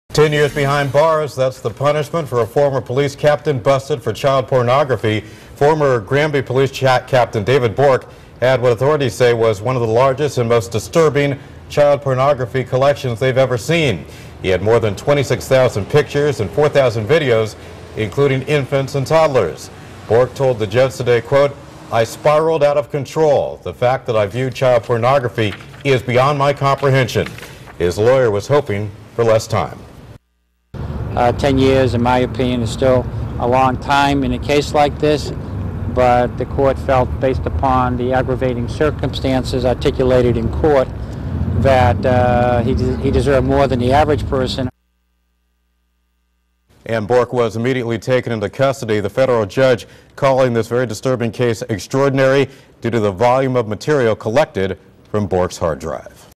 Former police captain to spend 10 years in jail
Former Granby Pd Captain David Bourque was sentenced to ten years in federal prison for possession of child pornography.